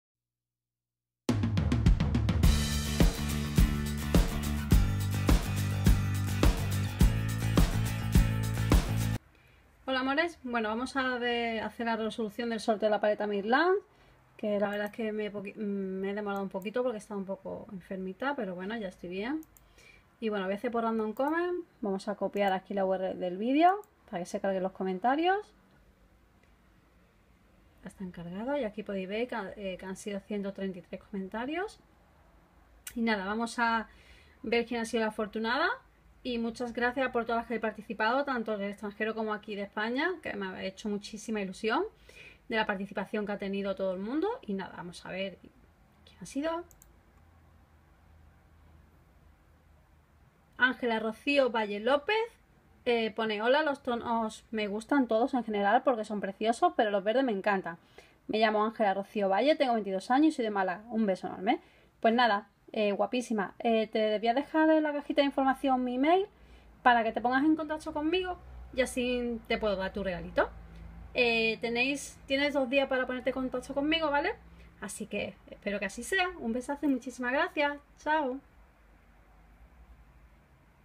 Resolución Sorteo Paleta Mirlans
La Ganadora por favor que se ponga en contacto conmigo por mail

Si en 2 días no hay noticias de ella, la volveré a sortear.

***CONECTA***

** Mi código en Iherb: HCJ123

"Este vídeo NO está patrocinado por nadie, todo lo que digo es mi propia opinión y experiencia"